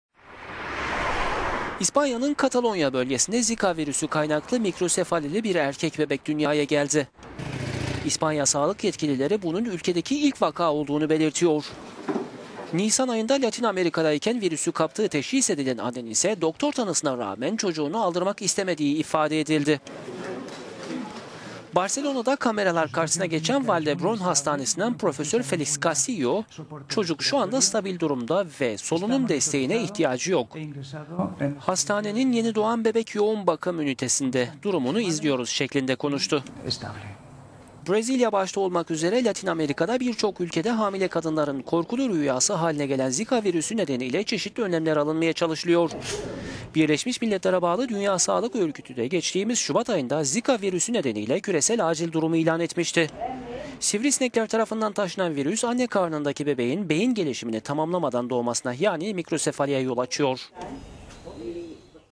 İspanya'da ilk mikrosefalili bebek dünyaya geldi
İspanya'nın Katalonya bölgesinde Zika virüsü kaynaklı mikrosefalili bir erkek bebek dünyaya geldi.

İspanya sağlık yetkilileri, bunun ülkedeki ilk vaka olduğunu belirtti.

Nisan ayında Latin Amerika'da iken virüsü kaptığı teşhis edilen annenin doktor tanısına rağmen çocuğunu aldırmak istemediği ifade edildi.

Barcelona'da kameralar karşısına geçen Vall D'Hebron Hastanesi'nden Profesör Felix Castillo, "Çocuk şu anda stabil durumda ve solunum desteğine ihtiyacı yok, hastanenin yenidoğan bebek y…